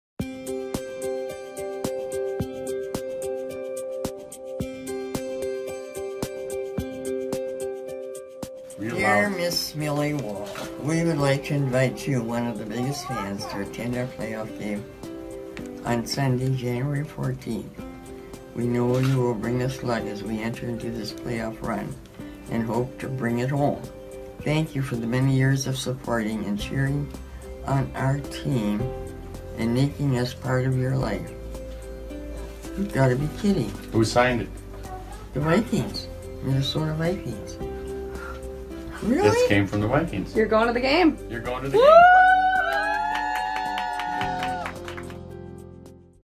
Vikings gift tickets to 99-year-old fan for first playoff game
Minnesota Vikings’ good luck charm? 99-year-old Millie Wall who will be attending her first playoff game.